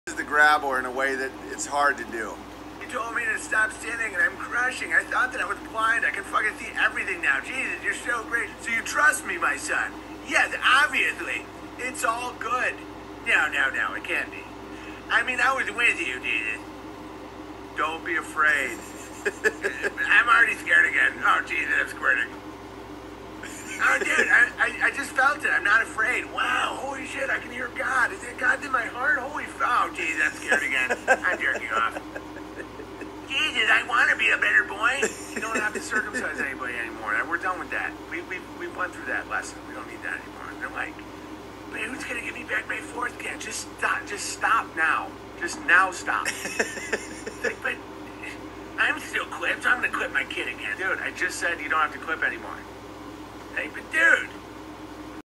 Jesus Meets The Grabbler
Clip from Big Bears recent stream where he shows Ira (The Grabbler) meeting the King of King’s and a hilarious conversation ensues.